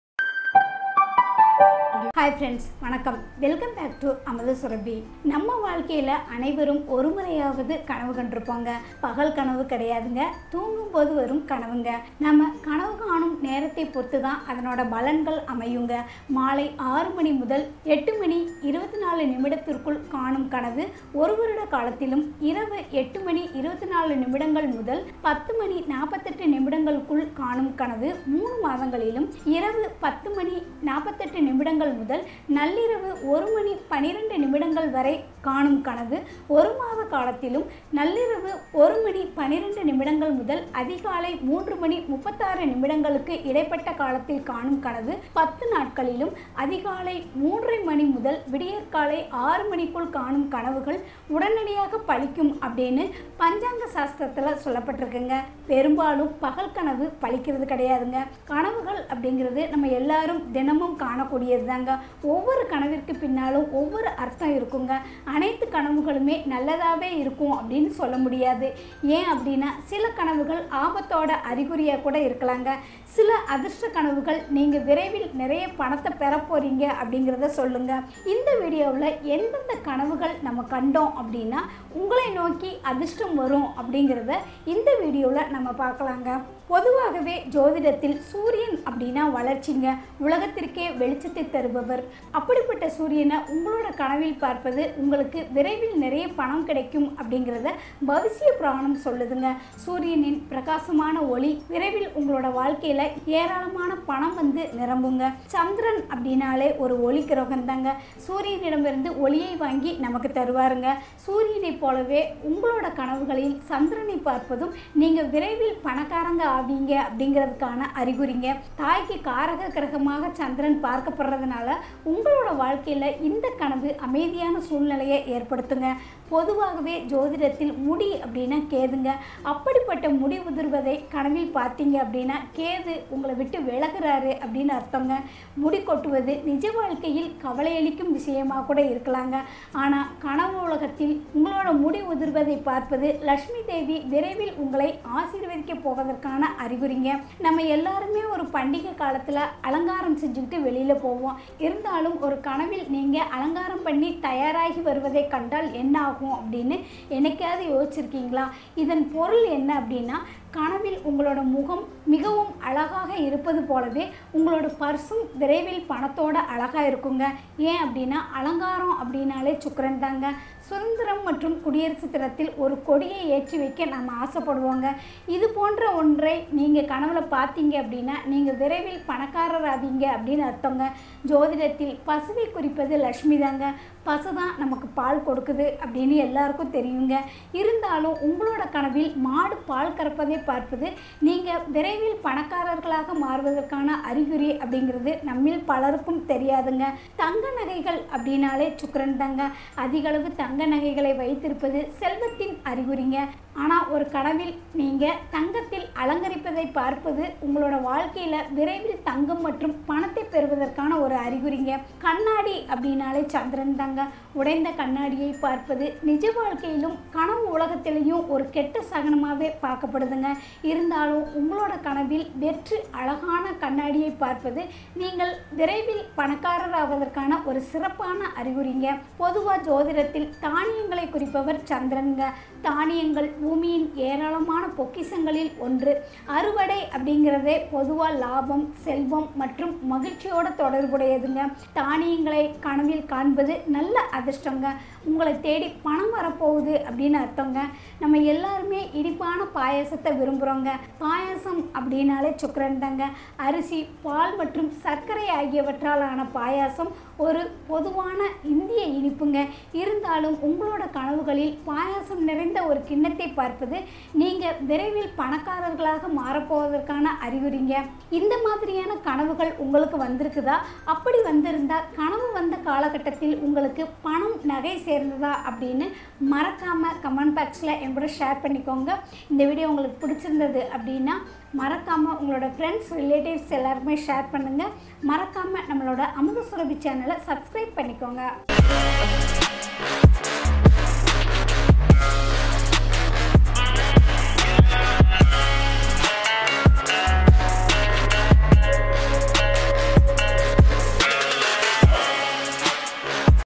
கனவுகள் இப்படி வந்தால் பணமும் அதிர்ஷ்டமும் தேடி வரும் | கனவு பலன்கள் | Kana | Kanavu Palan
கனவுகள் இப்படி வந்தால் பணமும் அதிர்ஷ்டமும் தேடி வரும்  | கனவு பலன்கள் | கனவில் | Kana | Kanavu Palan | Amutha Surabi

சில அதிர்ஷ்ட கனவுகள் நீங்கள் விரைவில் நிறைய பணத்தை பெறப்போகிறீர்கள் என்பதைசொல்லும்.. இந்த வீடியோவில் எந்தெந்த கனவுகள் உங்களை நோக்கி அதிர்ஷ்டம் தேடிவருகிறது என்பதை உணர்த்தும்  .